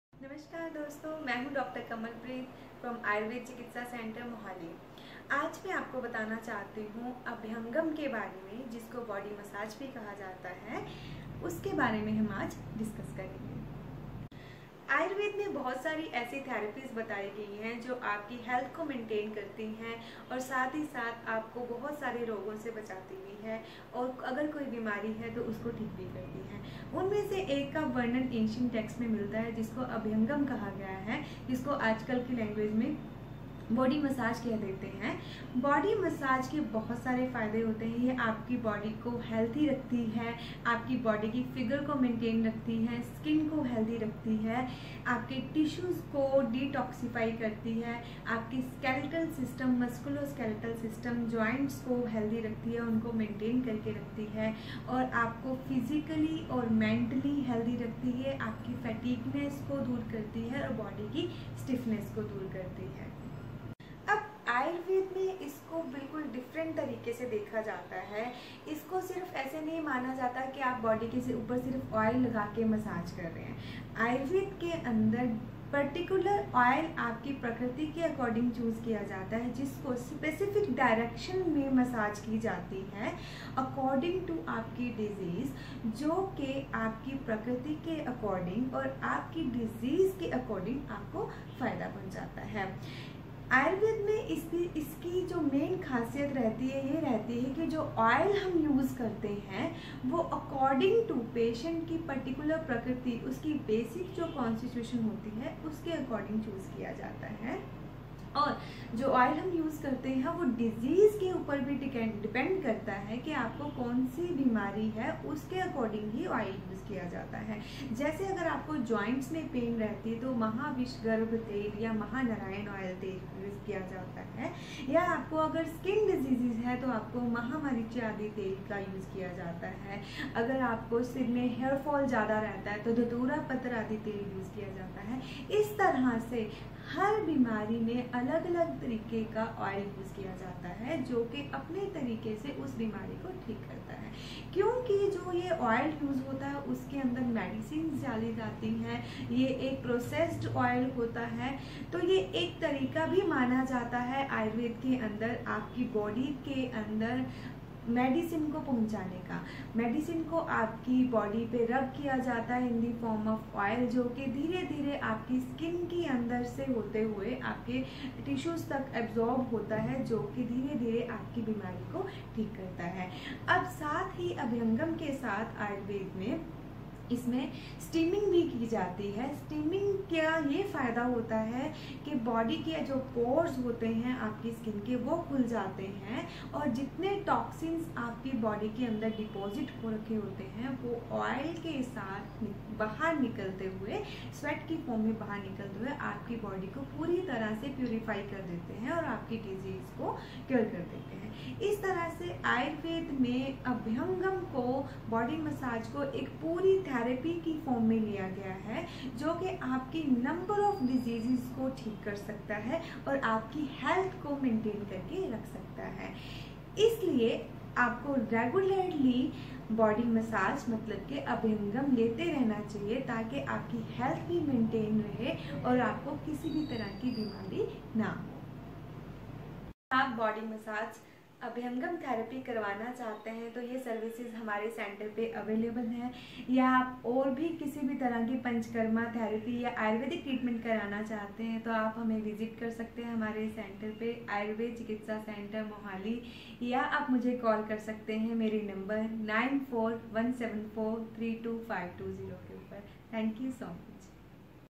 Sharir Abhayanga /Ayurvedic body Oileation and Massage
Sharir Abhayanga/ Ayurvedic Body massage*
Is oil application on body and massage in specific directions
 *BENEFITS*
.Improves blood circulation
.facilitates removal of toxins of tissues
.Relieves physical and mental fatigue
.Betters functioning  of musculoskeletal system
.Clears stiffness and heaviness in the body
.leads to feeling of lightness
.Assists development of healthy body
.Improvement in body figure
.Recuperates the body tissues
.Reduce body fat
.Has curative effects by pharmacological actions of drugs used in the process of the oil.
.Promotion and maintenance of health, as well as cure of illness of diseased.
 **HOW ABHYANGA IS DIFFERENT FROM BODY MASSAGE*
Abhyanga is not a simple procedure of oil and maneuvers
In it oil is used according to the prakriti(basic constitution) of a person
.Oil choosed according to diseases e.g. in Hairfall : dhaturaptradi tail,In skin diseases: Marichayadi tail,
Neurological disorders:Mahanarayana tail,
Sciatica: Maha visagarbha tail,joint diseases :karpoorafi taila
.This is a method for the drug administration.
So,found to be very much useful in curing many of diseases belonging to different systems of the body.
It helps in the treatment of vata vyadhi vis-a-vis neurological disorders.